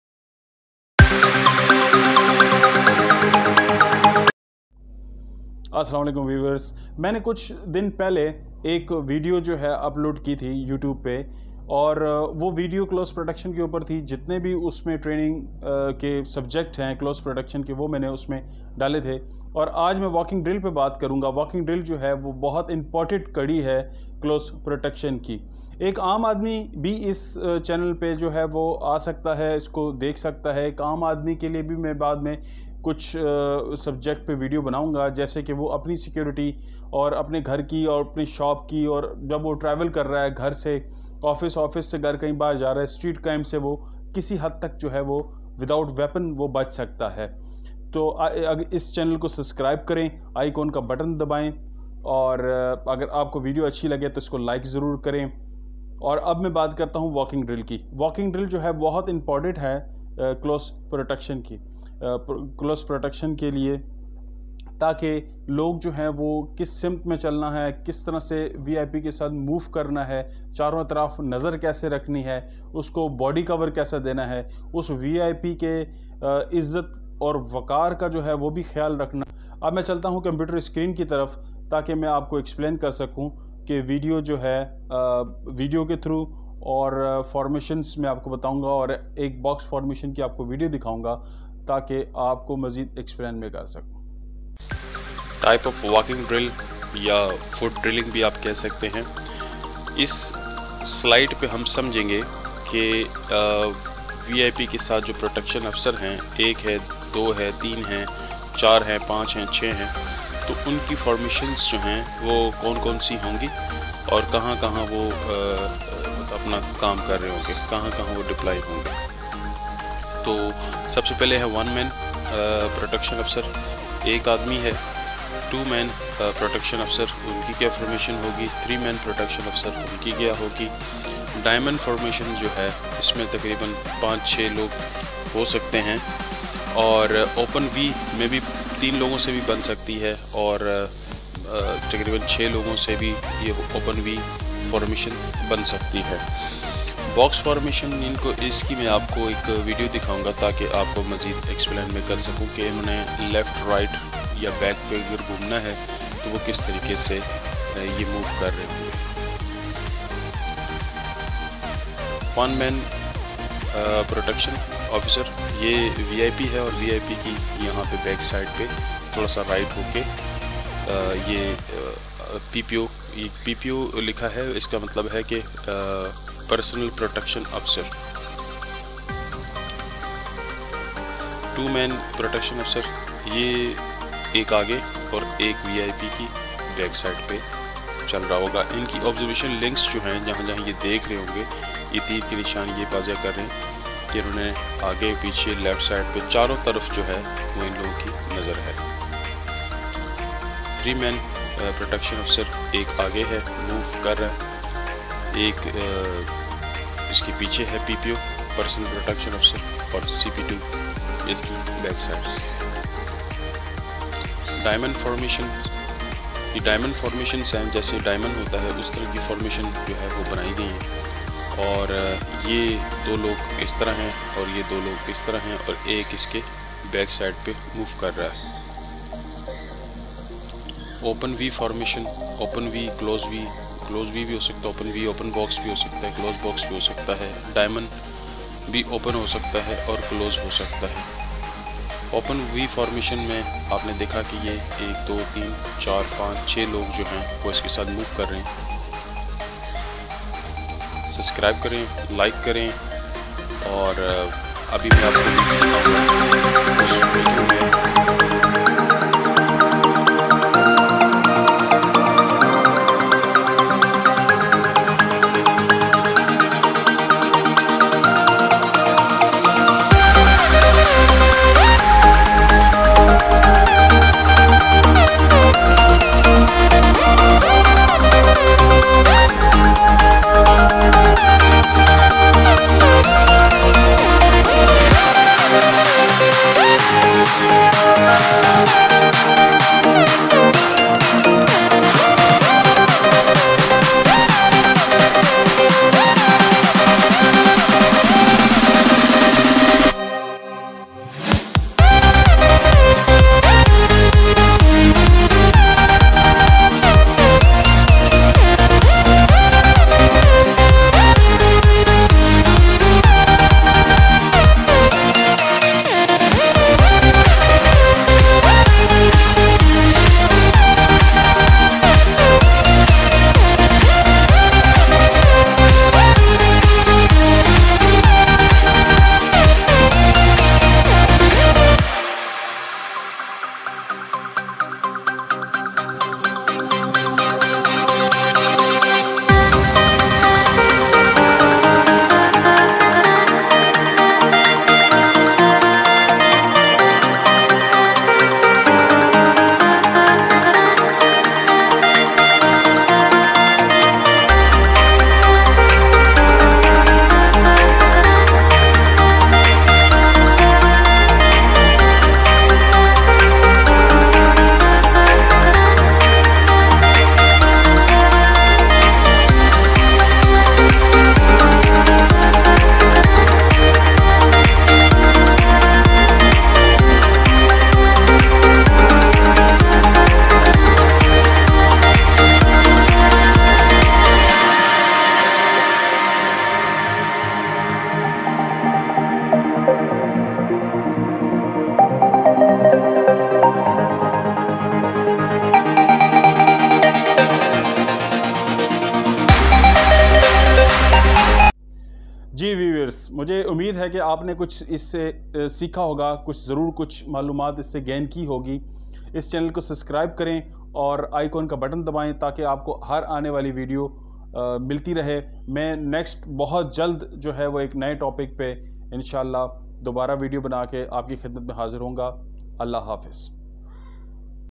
Close Protection Bodyguard Walking Drills
How Can You Secure Home Shop and Travel
learning about how can perform Security  duty with VIP and how many types of walking drills